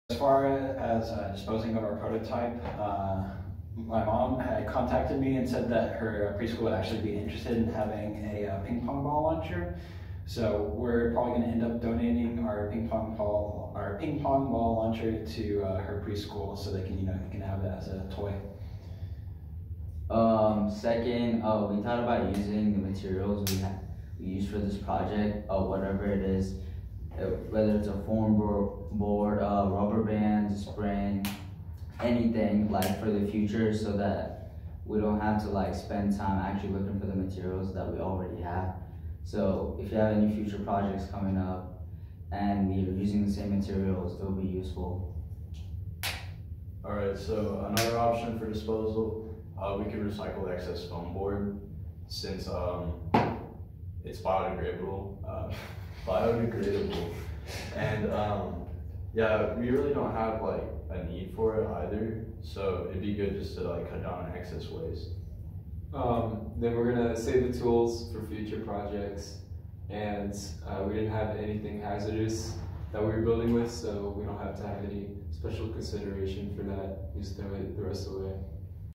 Ping Pong Ball Launcher Project Disposal
Discussion on our plans for disposing of and recycling the project.